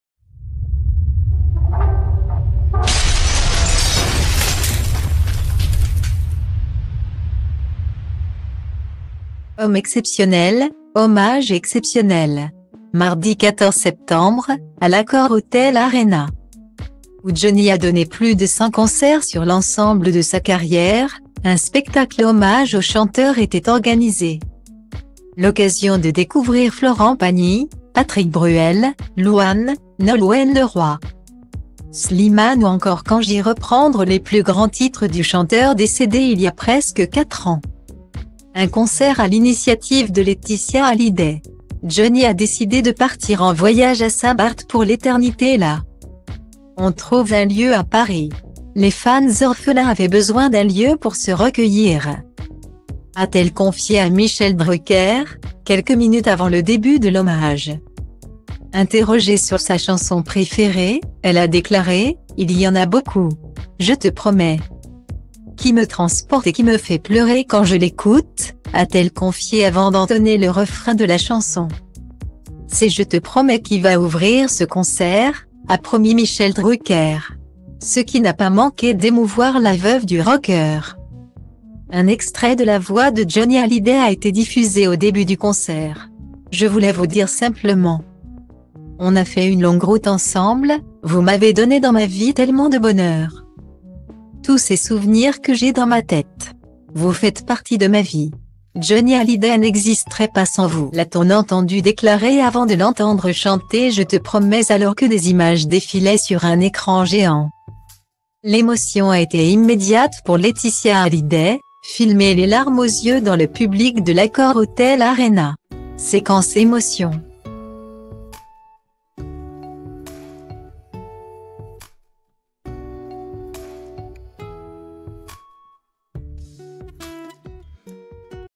VIDEO Concert hommage à Johnny : Laeticia en larmes dès les premières minutes
Homme exceptionnel, hommage exceptionnel. Mardi 14 septembre, à l'AccorHotel Arena,où Johnny a donné plus de cent concerts sur l'ensemble de sa carrière, un spectacle hommage au chanteur était organisé.L'occasion de découvrir Florent Pagny, Patrick Bruel, Louane, Nolwenn Leroy,